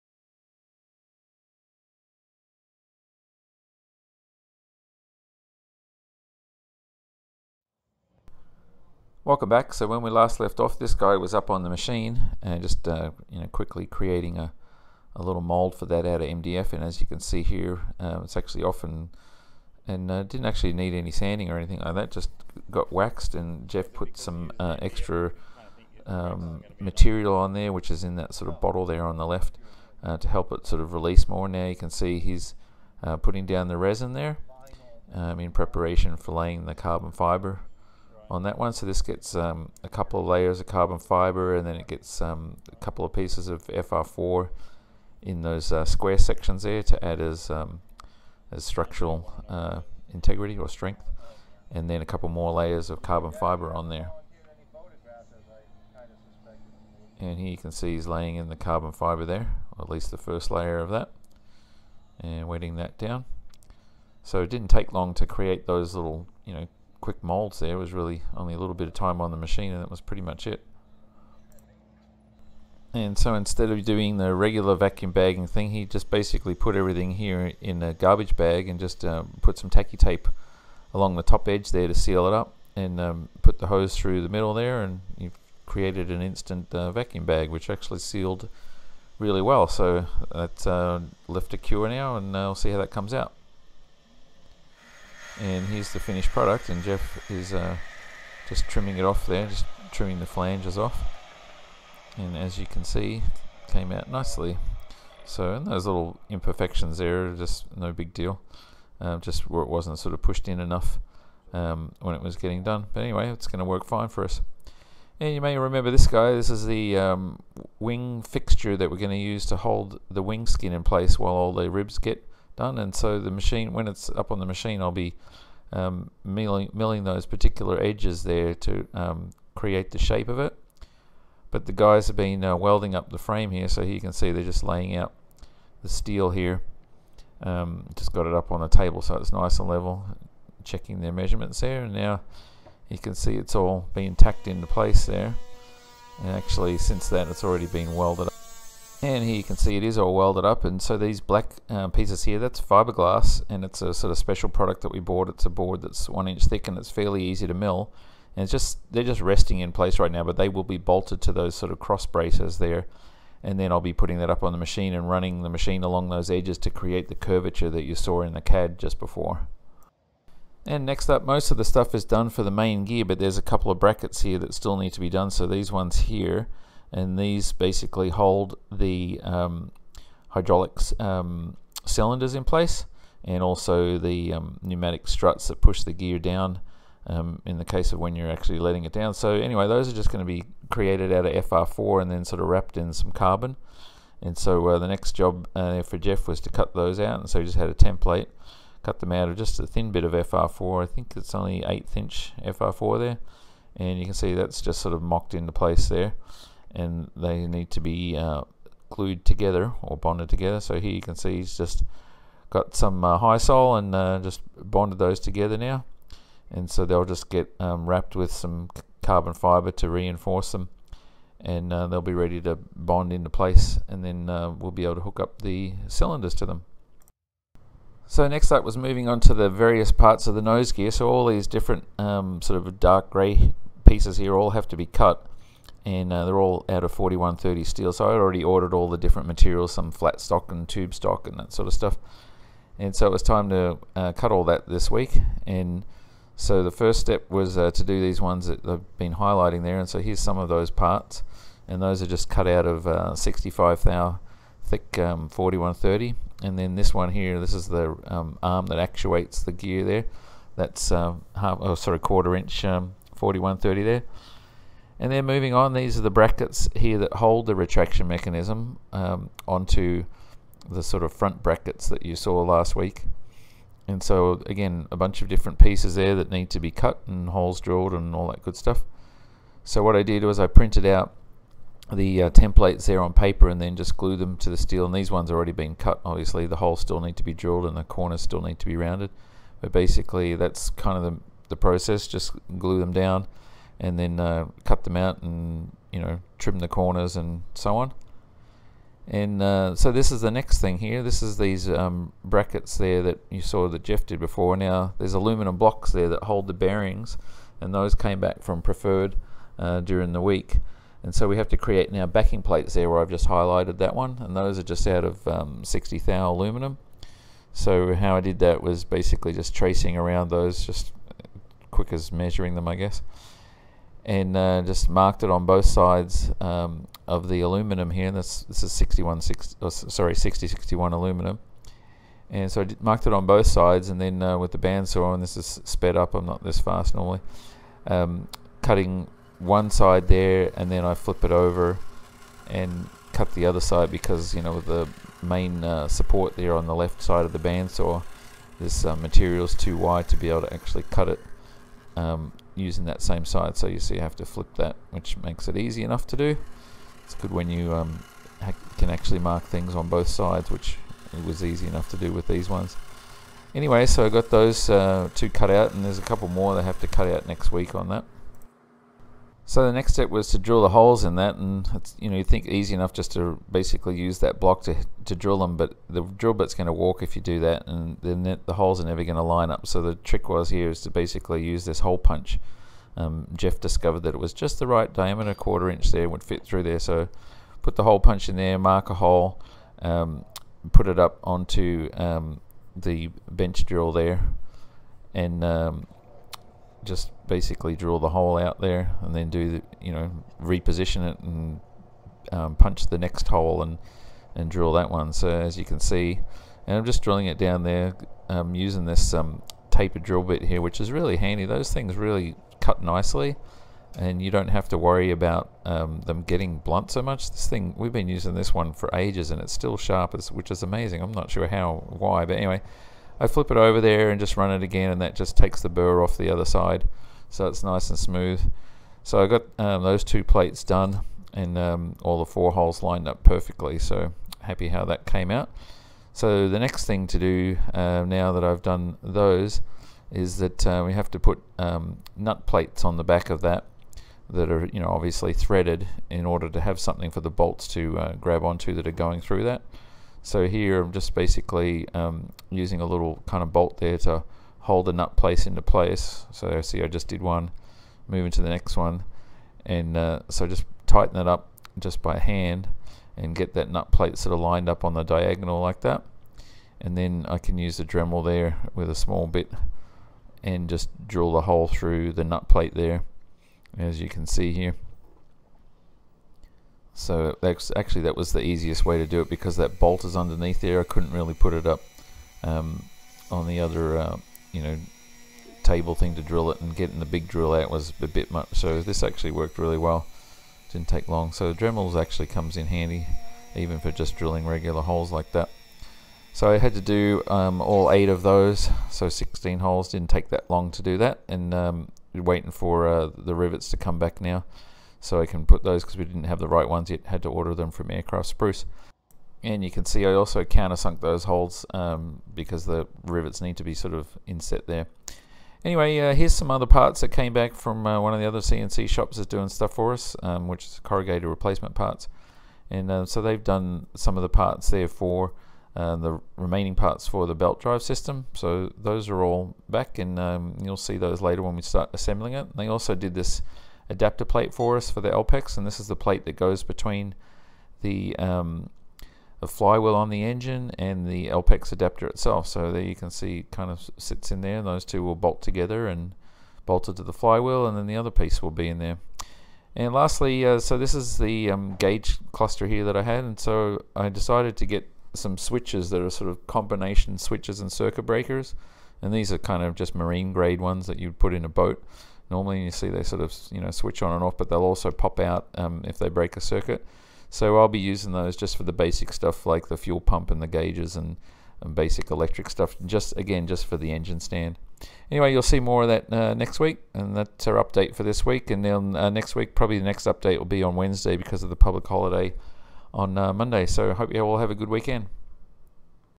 Shifting Into Gear - More work on the nose gear system
Cutting the 4130 for the parts of the nose gear retraction system. Laying up the nose gear brackets. Work on the wing fixture.